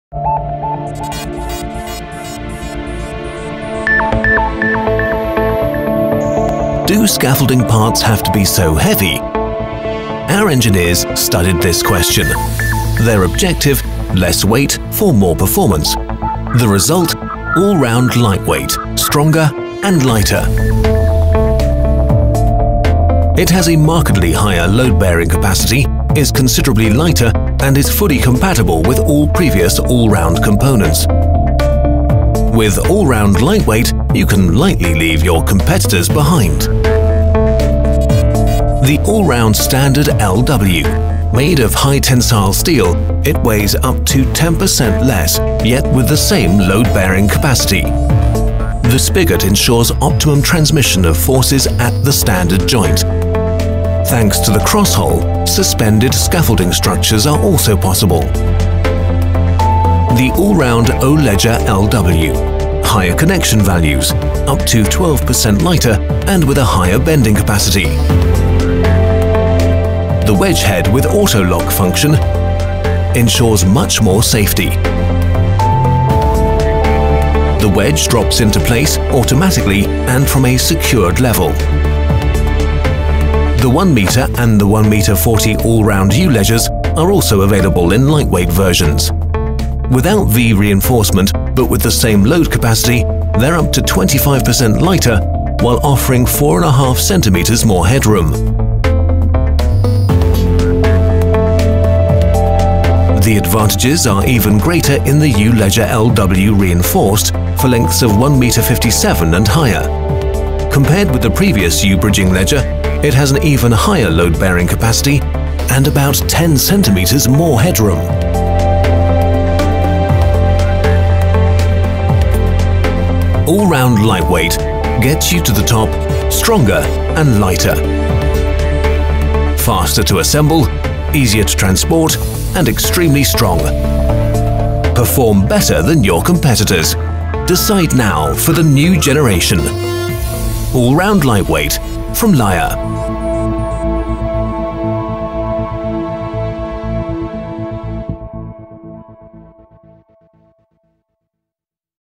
Layher Allround Scaffolding Lightweight - The new generation
Fast.
The lighter components increase the speed of assembly and also transport capacities – that saves real money.

Strong.
Increased loading capacity despite lower weight.

Safer.
Improved occupational safety during assembly thanks to the unique AutoLock function.

Ergonomic.
Increased height clearance and reduction of physical strain thanks to the weight saving in every part.

Efficient.
Only one standard for supported and suspended scaffolding thanks to integrated spigots. There is no need for time-consuming installation of separate spigots or storage of two different standards.